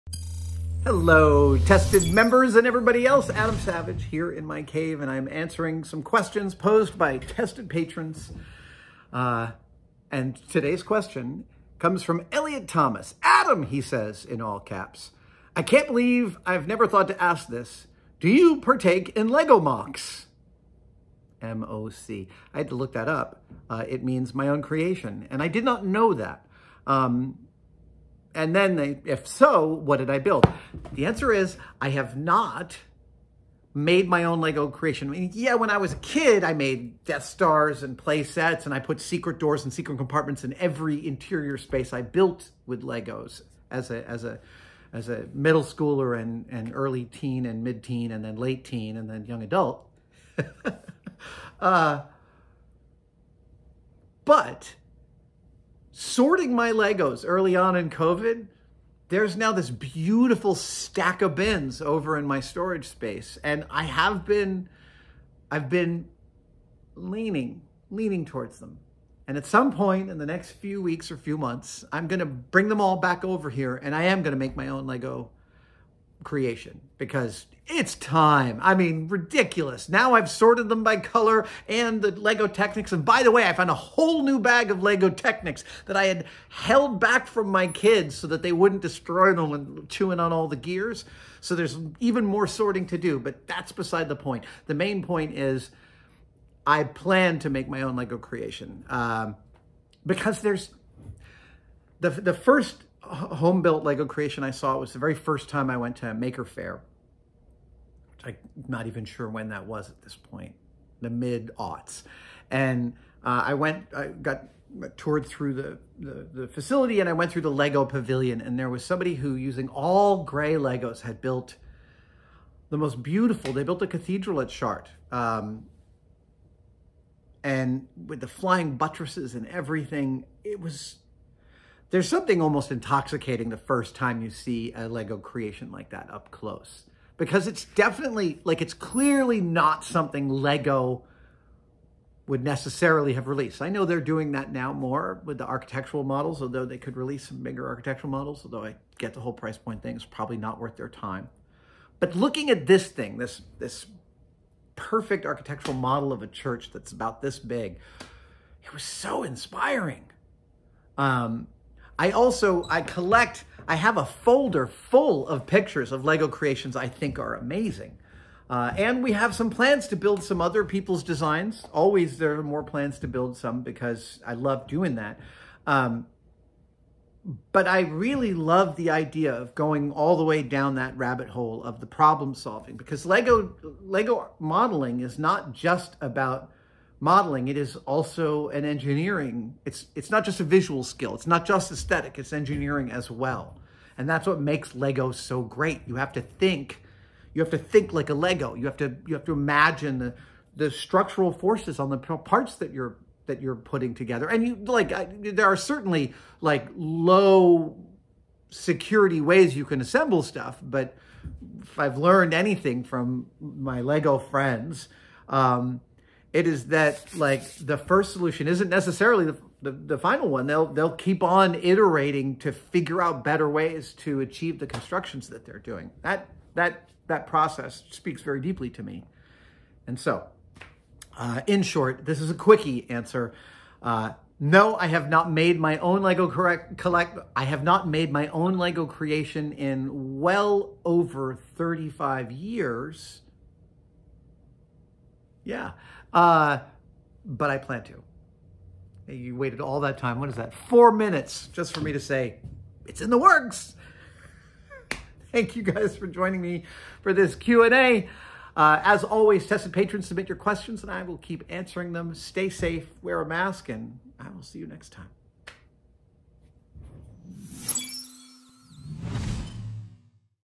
Ask Adam Savage: LEGO MOCs
This question from Tested Patron Eliot Thomas required Adam to do a quick Google search, but he loved the idea! Here's the FULL question: "ADAM! I can't believe I've never thought to ask this: Do you partake in Lego MOCs? If so, what's a favorite you've built, and what's something you've always wanted to build or see built?" Thanks for your question and support, Eliot!